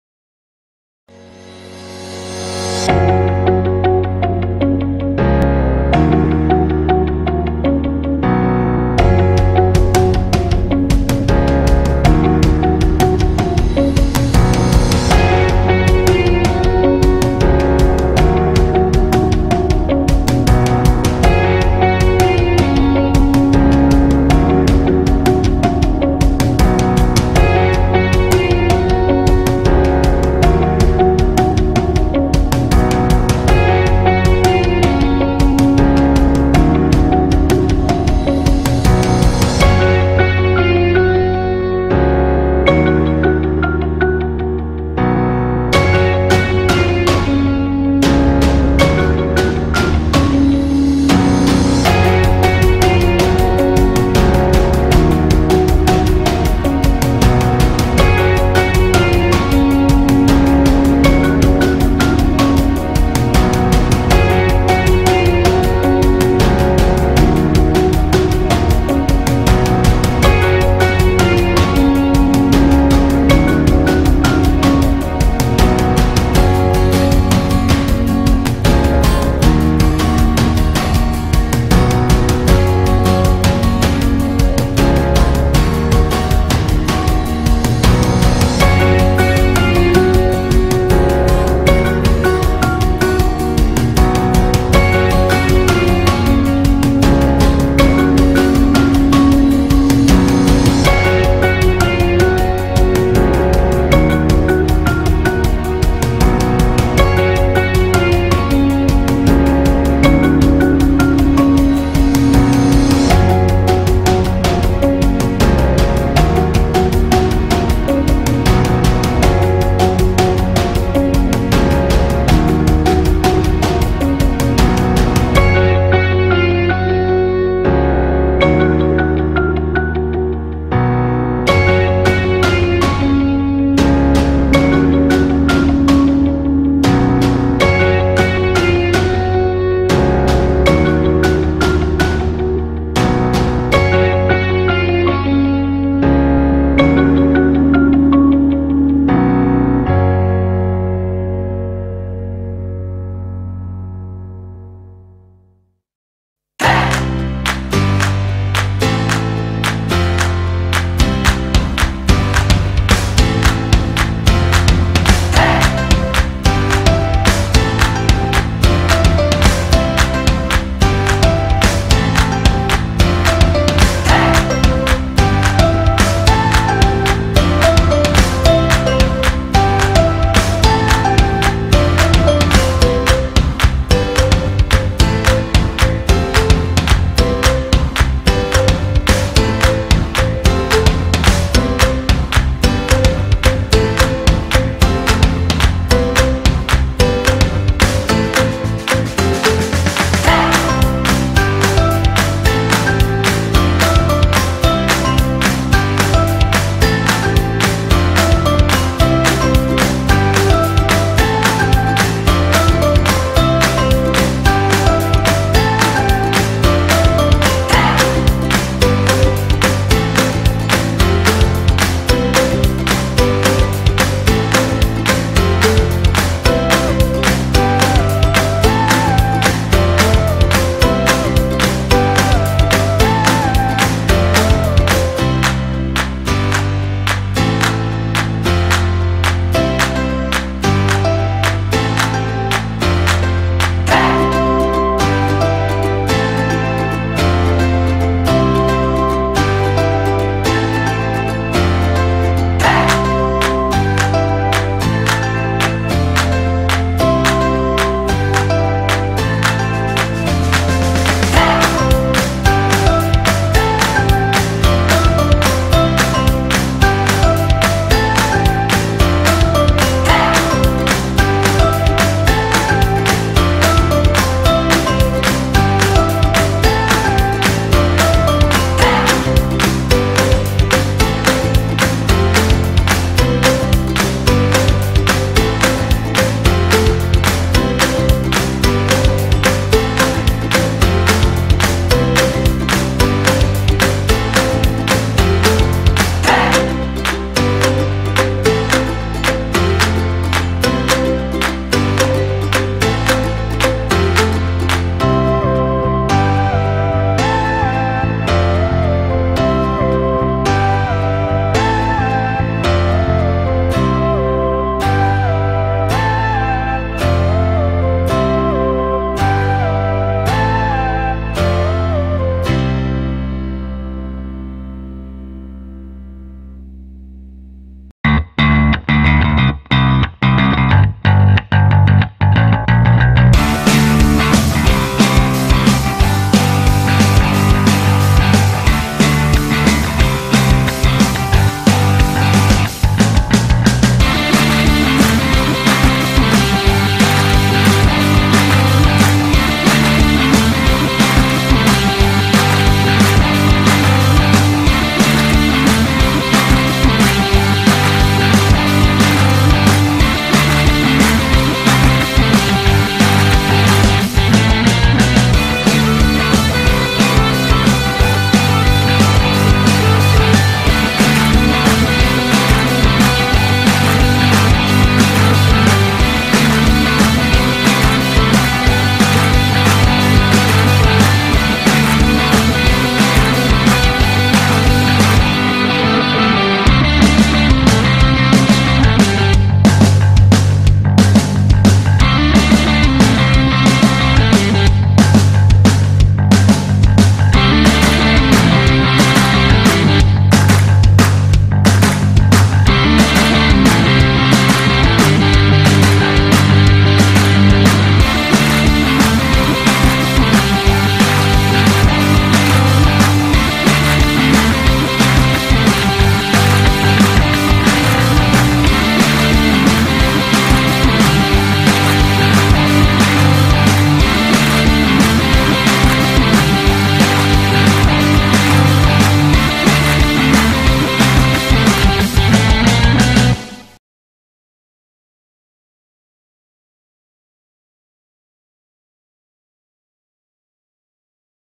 Building A Thatch Palapa
Watch a 3 day build via stills and time-lapse video